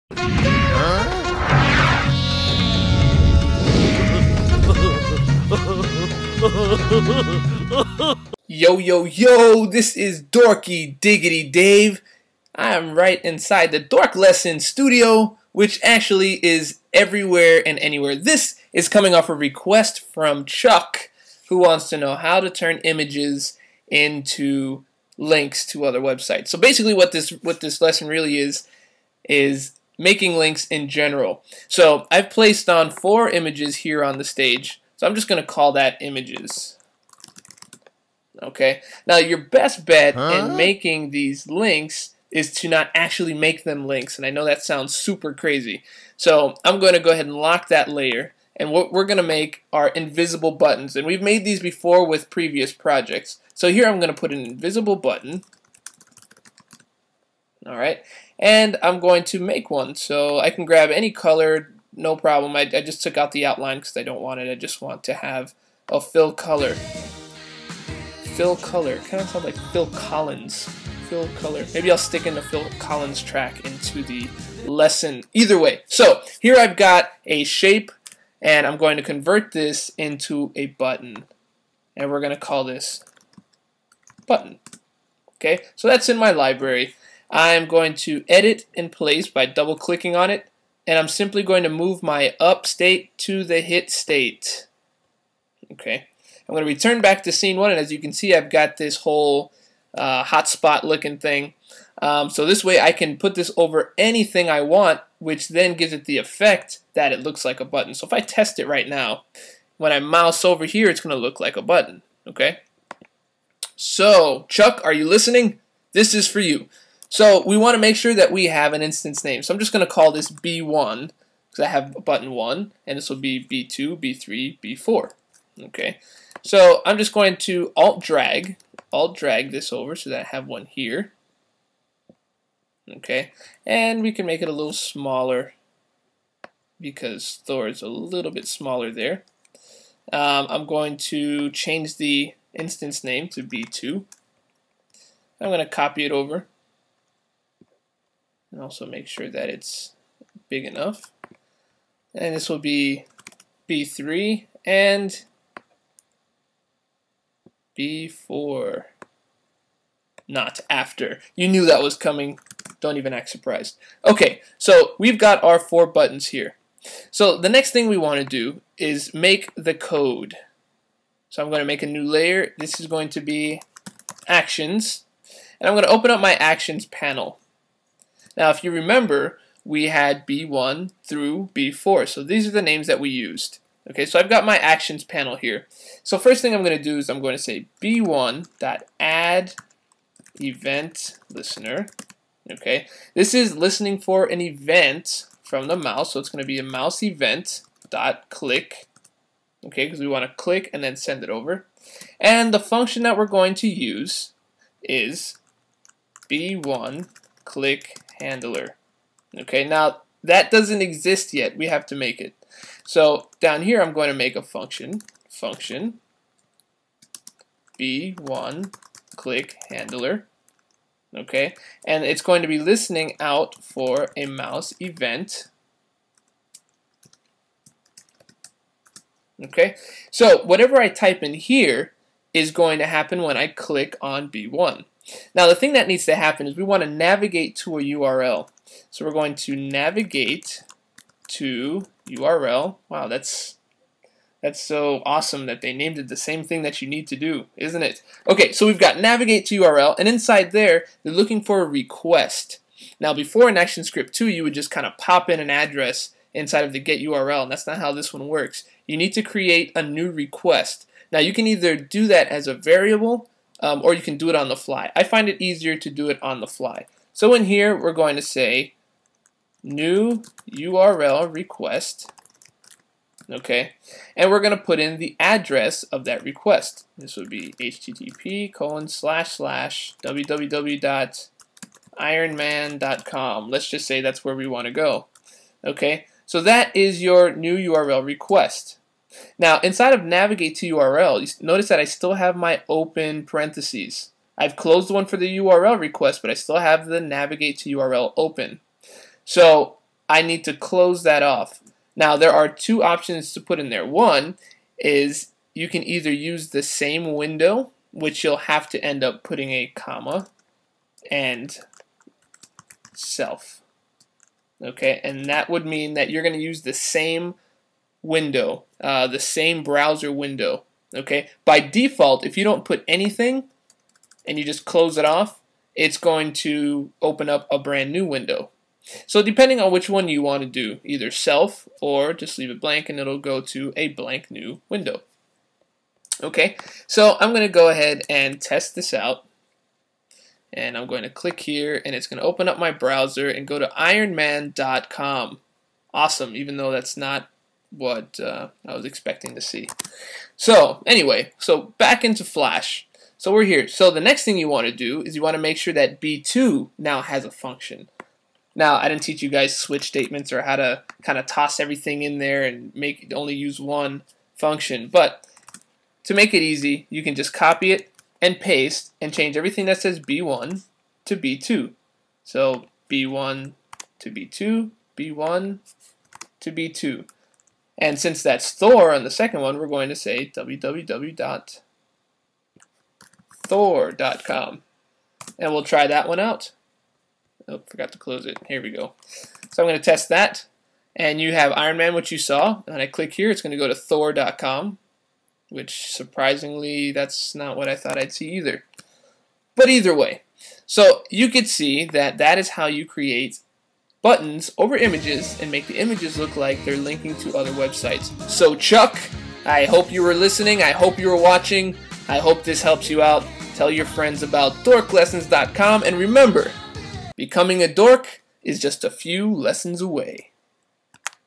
Creating Links to Websites in Flash CS3
In this Tutorial you will learn how to navigate to an External URL using Action Script 3.  Also how to make invisible buttons to be reused over multiple images.